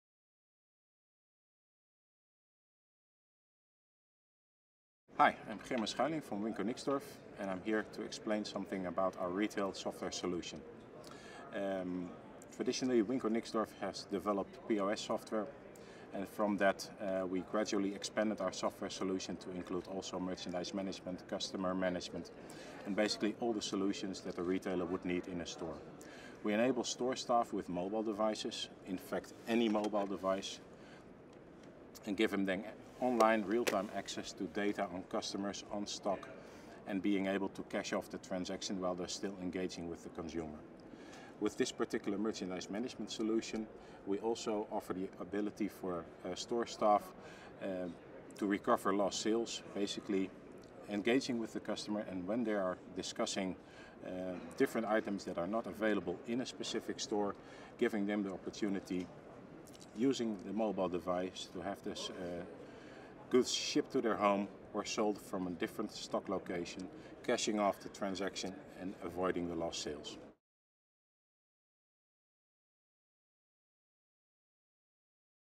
Zebra Technologies: Merchandise Management with Wincor Nixdorf, NRF 2016
Zebra's Merchandise Management Solutions - either in the hands of the store staff or in the hands of the customer – are playing a decisive role in enabling shopping experiences, as well as optimizing operational costs and increasing revenue.  Learn more about Zebra's Merchandise Management Solutions by visiting